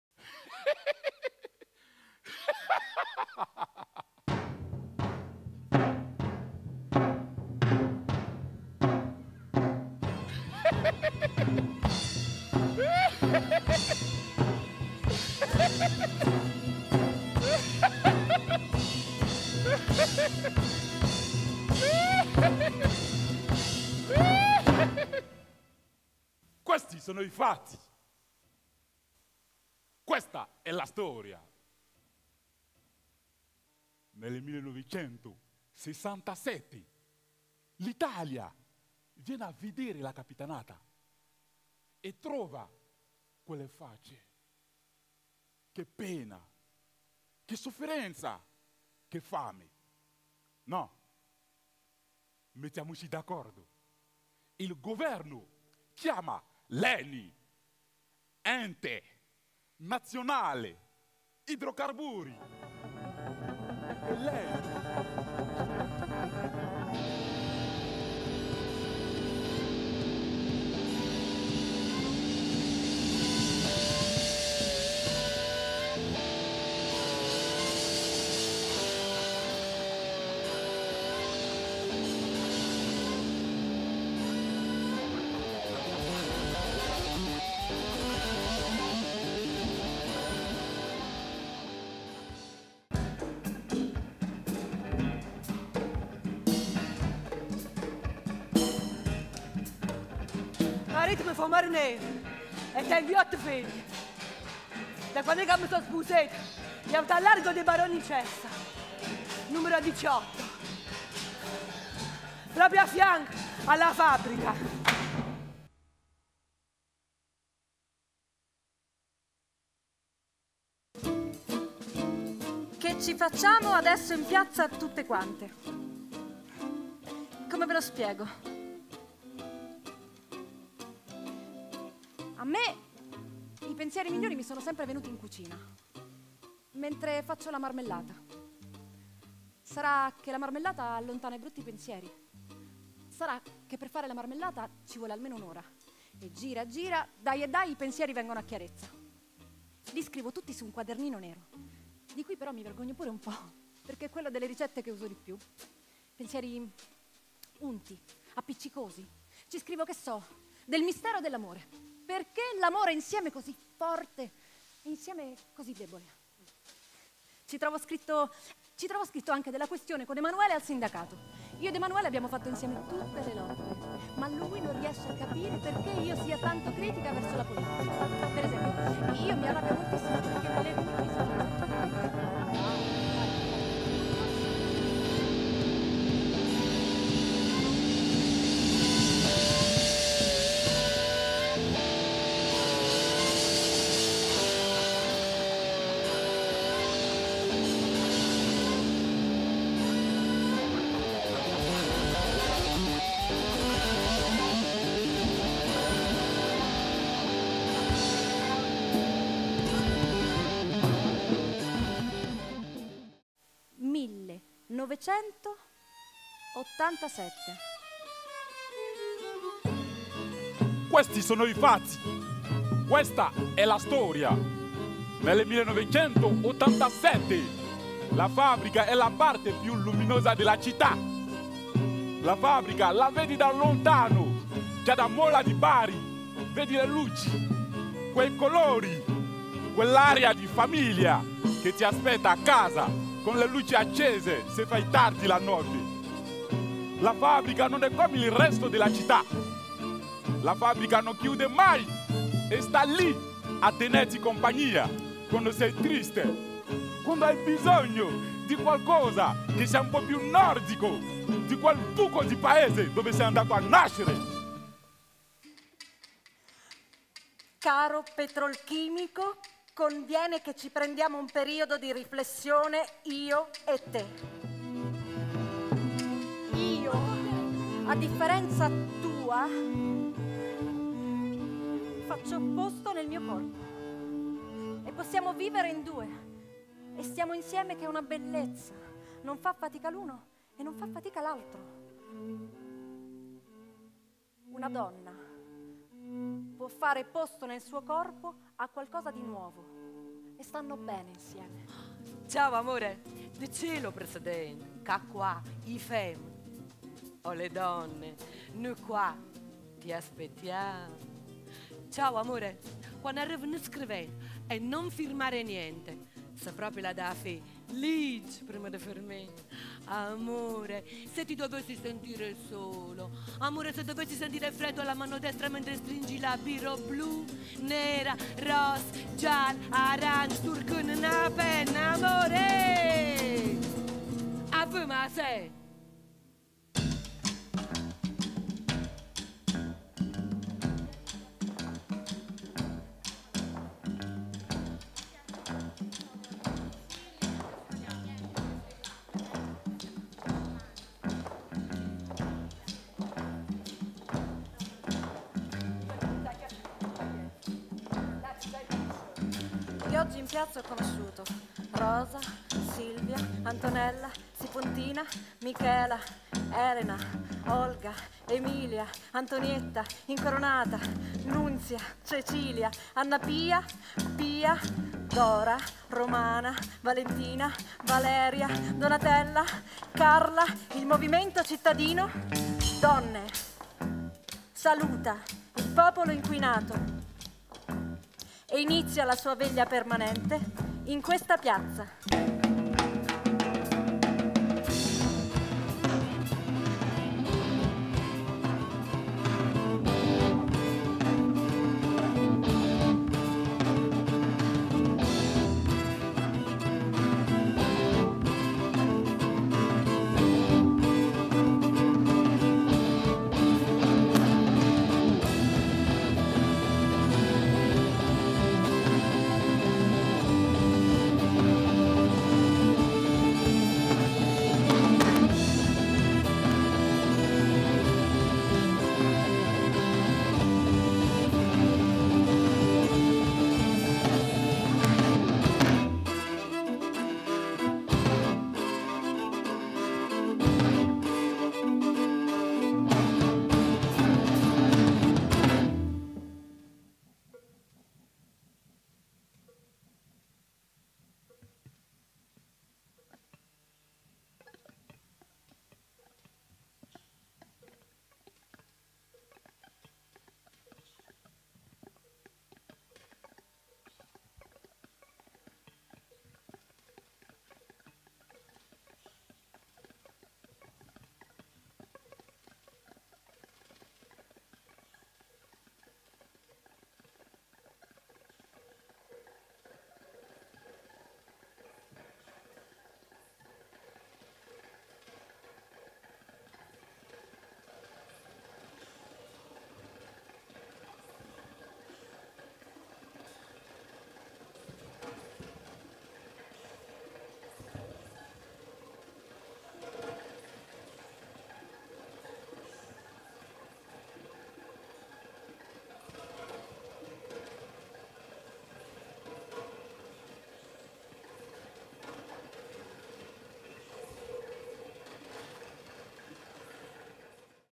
SottoSopra la città salvata dalle donne e altri scherzi simili
Da una idea di Cosimo Severo, Stefania Marrone. Drammaturgia di Stefania Marrone, Musiche di Fabio Trimigno,
Rregia Cosimo Severo.
Con Franck Nassirou, Livia Gionfrida, Nunzia Zoccano, Raffaella Giancipoli, Antonella Pazienza, 15 donne della città di Manfredonia.
Musicisti in scena Vincenzo Starace, Luca Lalla, Fabio Trimigno, Vincenzo Scarpiello.
Disegno luci Pino De Luca
Spazio sonoro e fonico Danilo Mottola
produzione Bottega degli Apocrifi, con il sostegno di Regione Puglia, Ministero Beni e Attività Culturali.
Anno 2007.